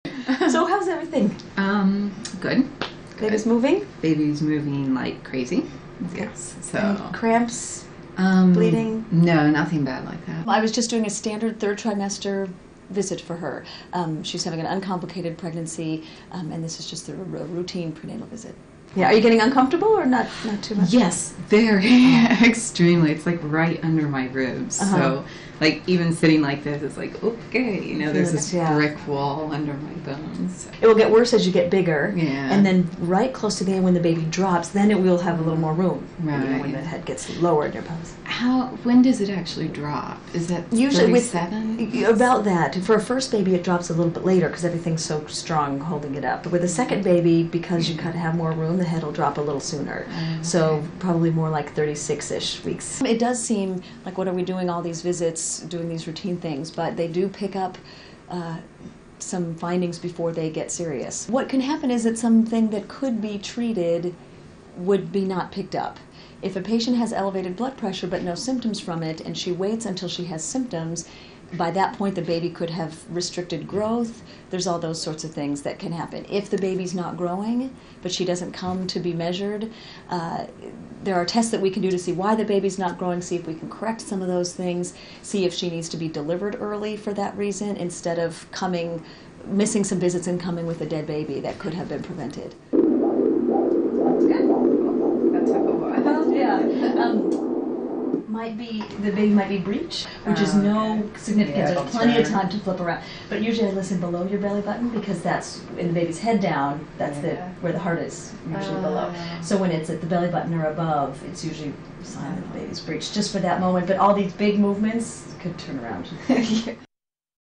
Prenatal visits
Why is it necessary to go for regular prenatal checkups? Professor Maura Quinlan advises all women to keep these appointments because they can reveal life threatening problems even before the woman realises anything is wrong.